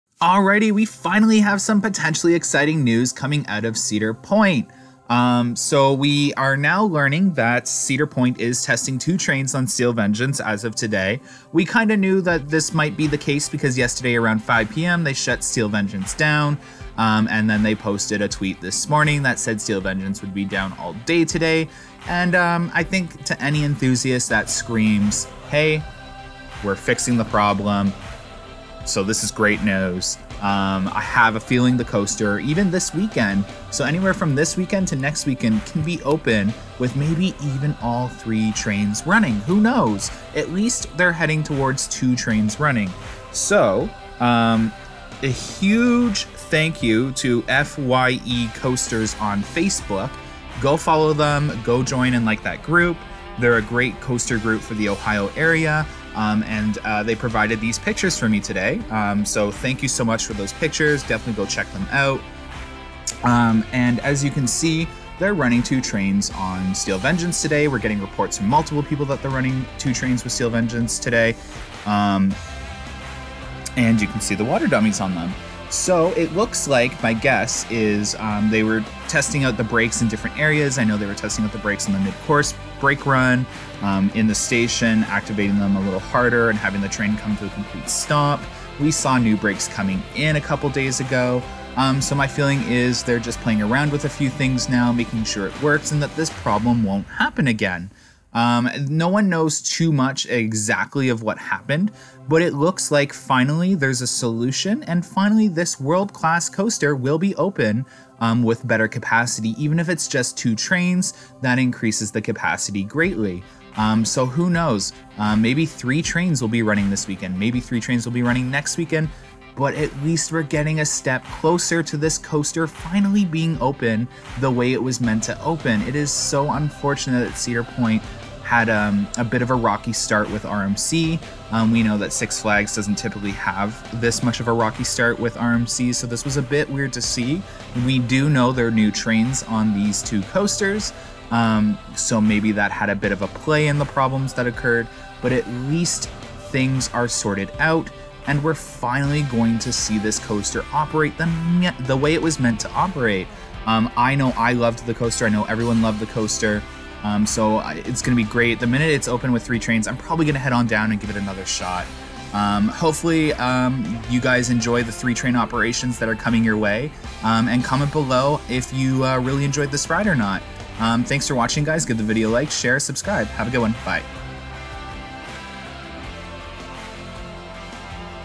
Steel Vengeance 3 Train Operations?
Join us as we discuss the potential for 3 train operations starting this weekend or next!